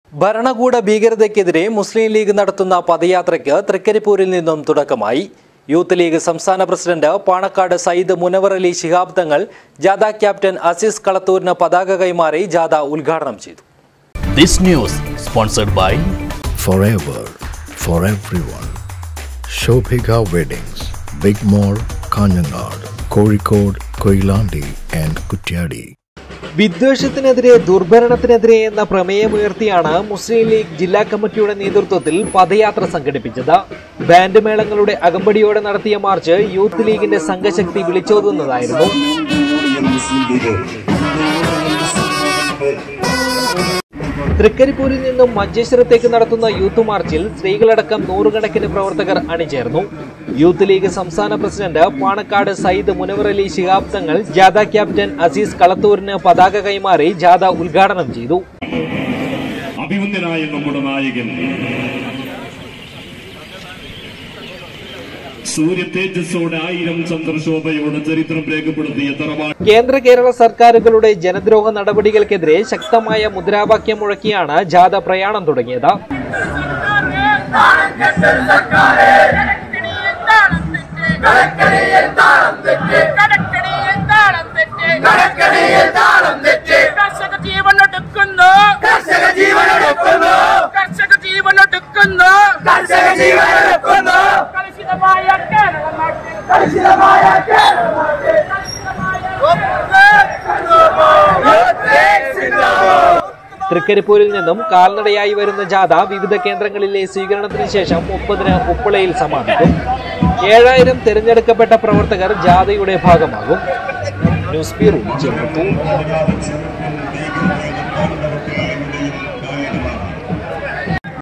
ഭരണകൂട ഭീകരതയ്‌ക്കെതിരെ മുസ്‌ലീം ലീഗ് നടത്തുന്ന പദയാത്രയ്ക്ക് തൃക്കരിപ്പൂരില്‍ നിന്നും തുടക്കായി.
ഭരണകൂട ഭീകരതയ്‌ക്കെതിരെ മുസ്‌ലീം ലീഗ് നടത്തുന്ന പദയാത്രയ്ക്ക് തൃക്കരിപ്പൂരില്‍ നിന്നും തുടക്കായി. യൂത്ത് ലീഗ് സംസ്ഥാന പ്രസിഡന്റ് പാണക്കാട് സയ്യിദ് മുനവറലി ശിഹാബ് തങ്ങള്‍ ജാഥാ ക്യാപ്റ്റിയന്‍ അസീസ് കളത്തൂരിന് പതാക കൈമാറി ജാഥ ഉദ്ഘാടനം ചെയ്തു.